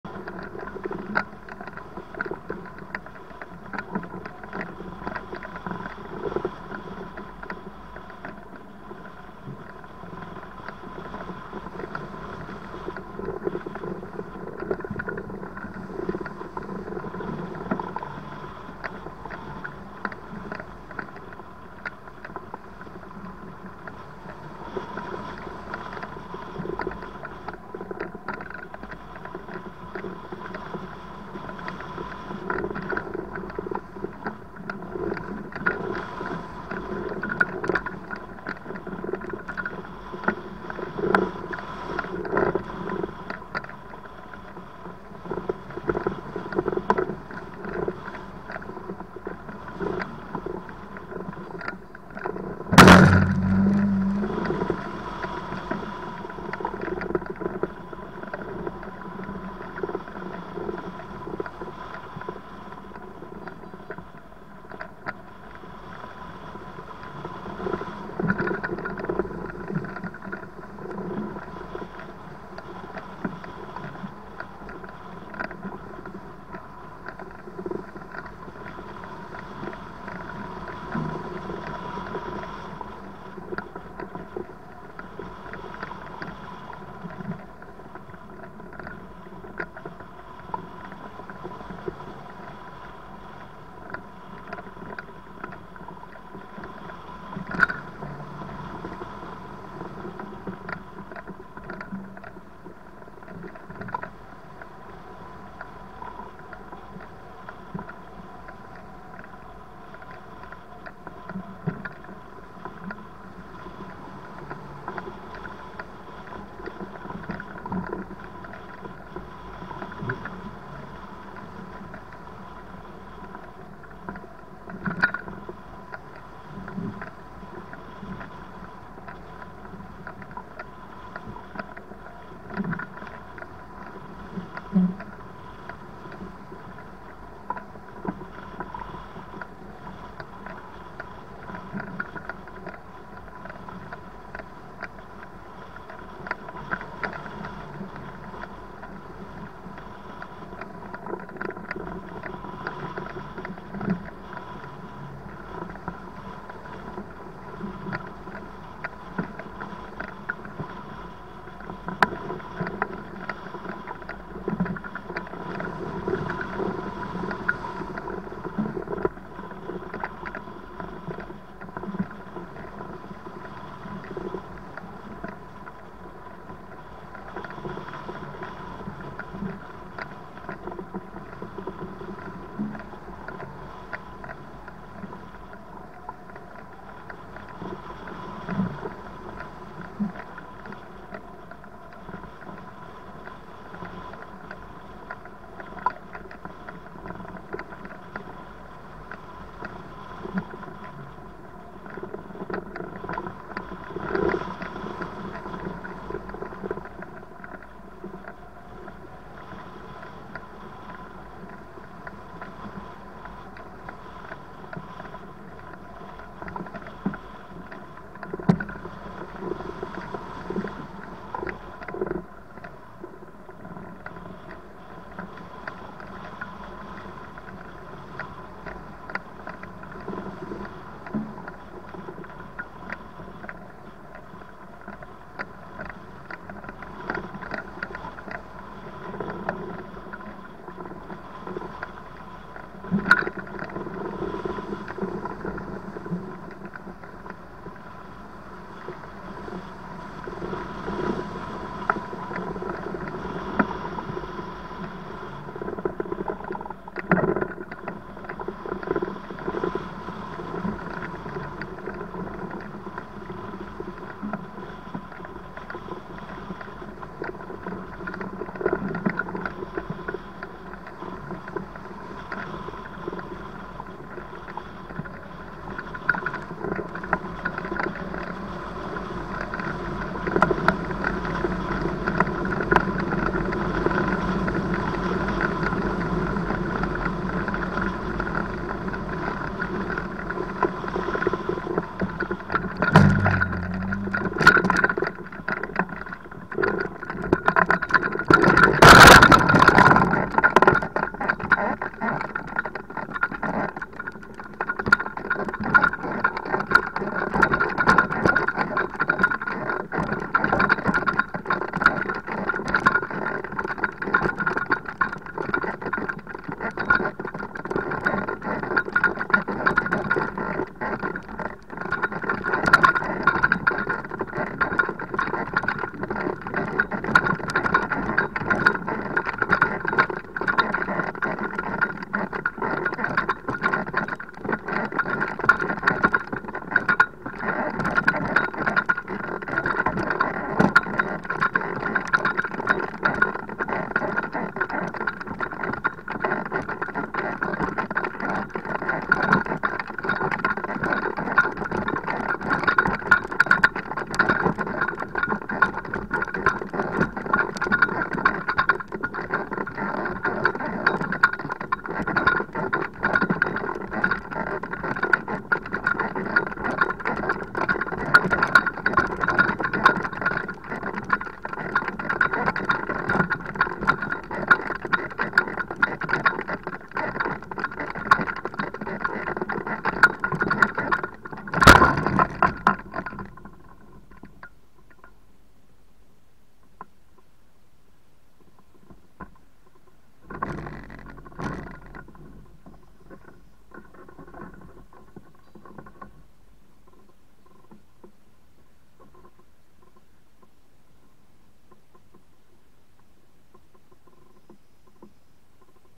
Bartolo 10/9/2022 Landing
Can't get myself back up the hill, go for landing then. Upslope landing, little tailwind and all.

Icaro2000 RX2 18L